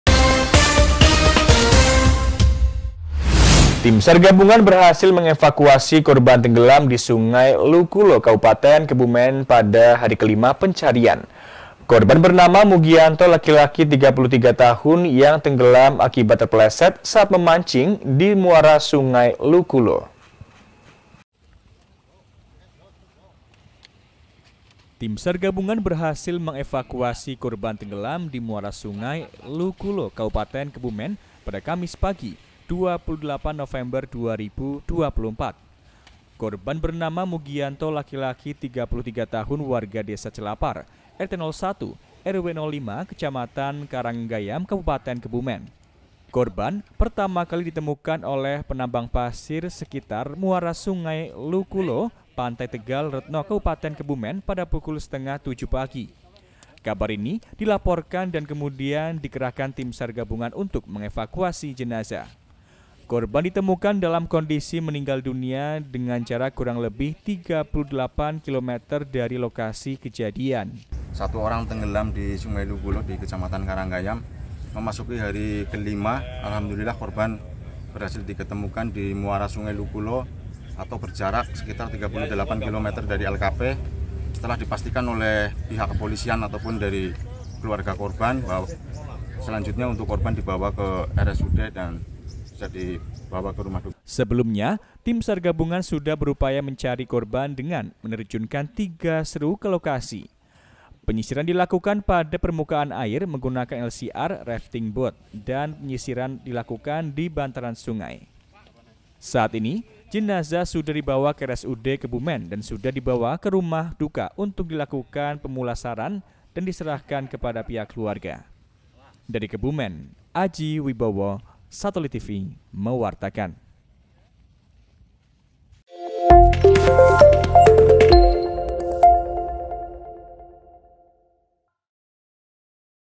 HARI KELIMA PENCARIAN, PEMANCING TENGGELAM DI SUNGAI LUKULO DITEMUKAN
Tim Sar gabungan berhasil mengevakuasi  korban tenggelam di Sungai Lukulo Kabupaten Kebumen pada hari ke 5 pencarian. Korban bernama Mugiyanto laki laki 33 tahun yang tenggelam akibat terpeleset saat sedang memancing di Muara Sungai Luku Lo.